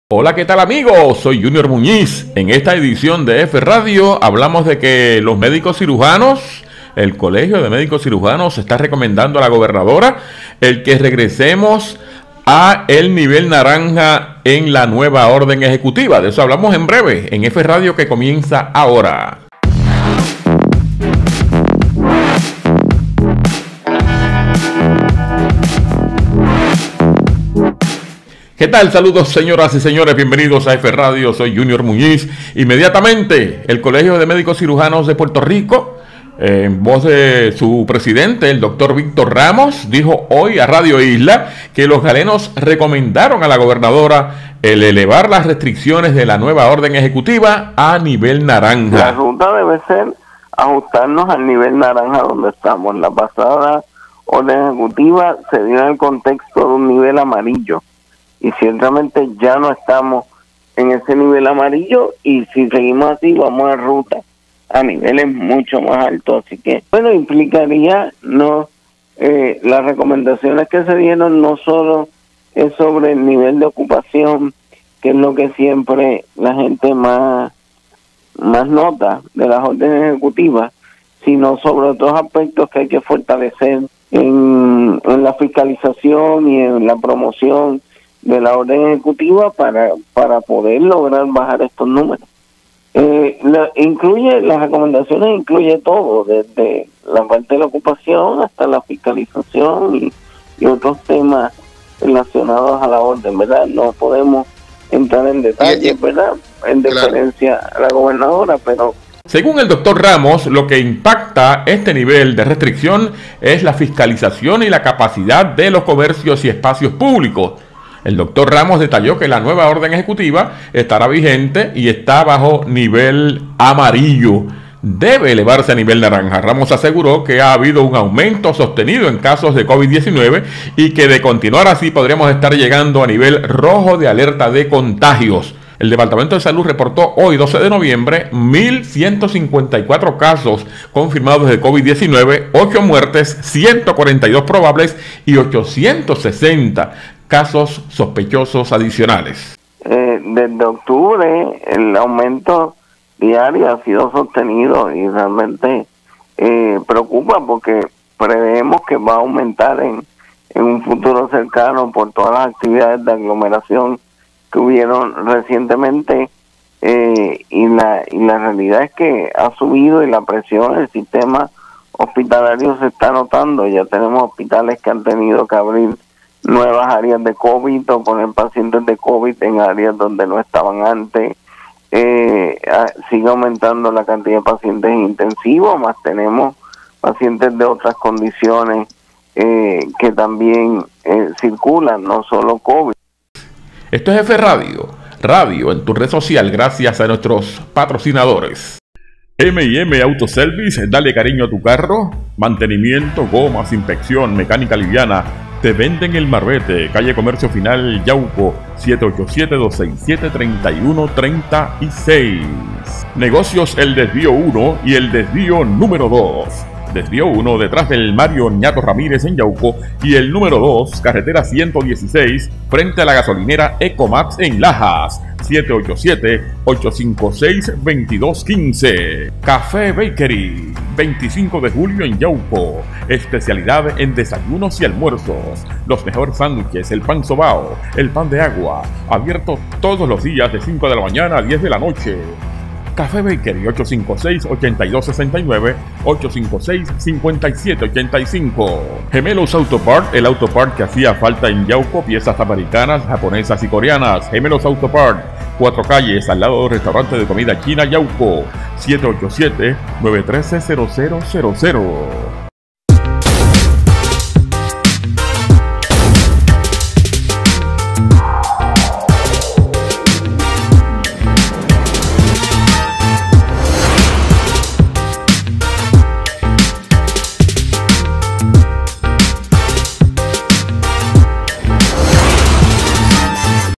FRadioPodcast - El Colegio de medicos recomienda regresar a alerta Naranja
FRadioPodcast - El Colegio de medicos recomienda regresar a alerta Naranja en l Naeva Orden Ejecutiva